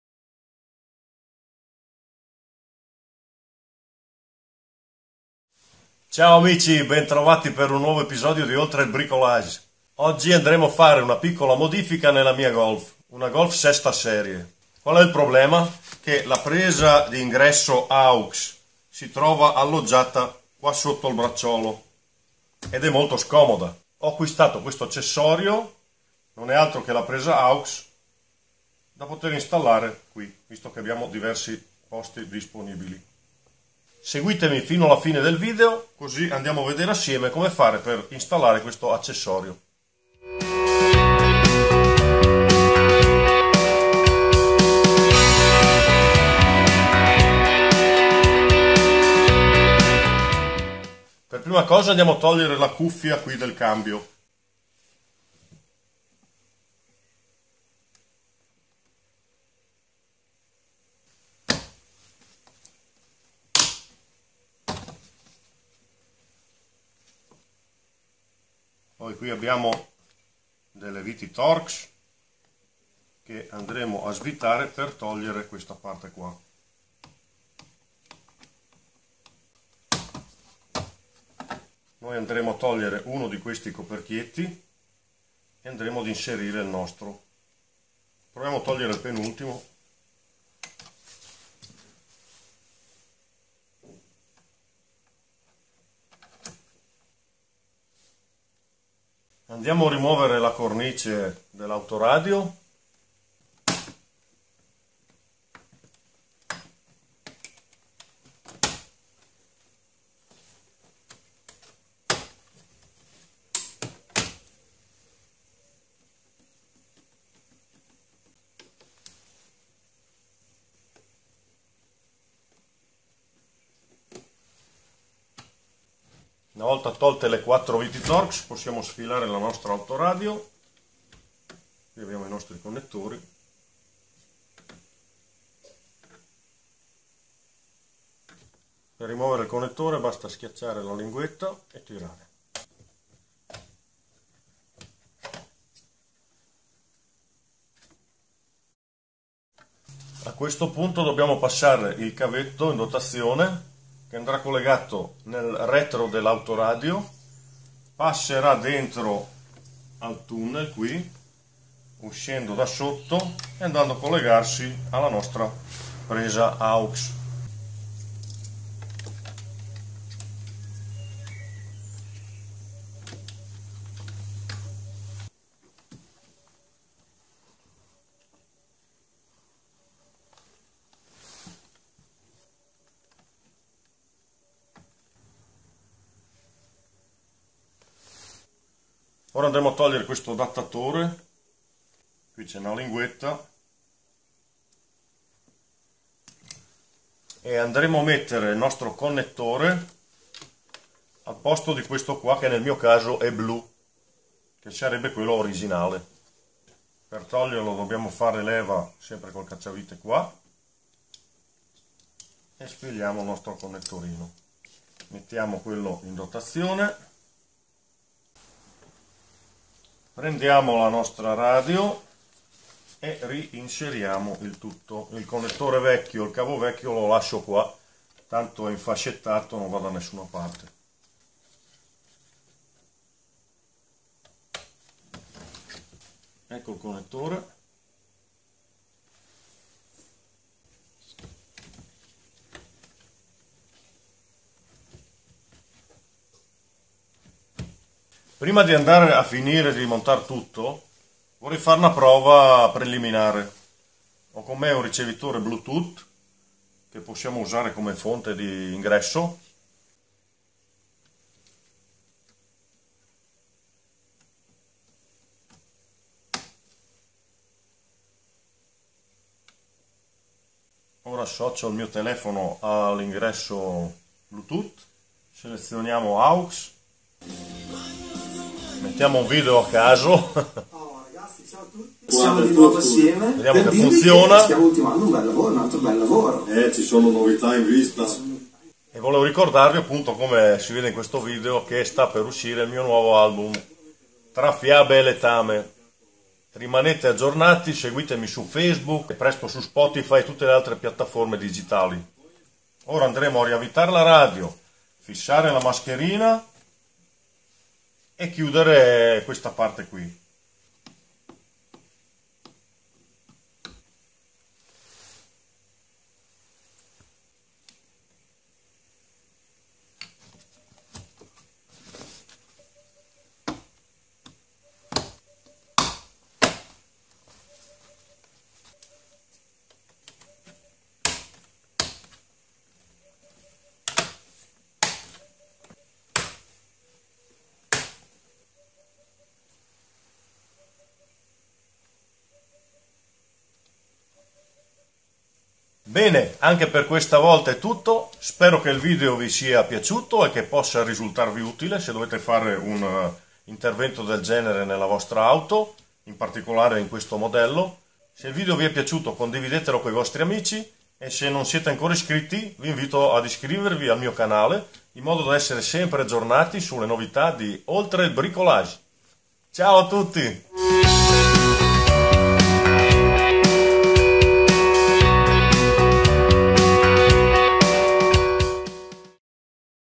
Video 48 - Installazione presa AUX IN su VW Golf VI
Ciao amici, volevo condividere con voi questo breve video in cui installo, con pochi semplici passaggi, una presa AUX IN nella Golf VI, sperando possa esservi utile.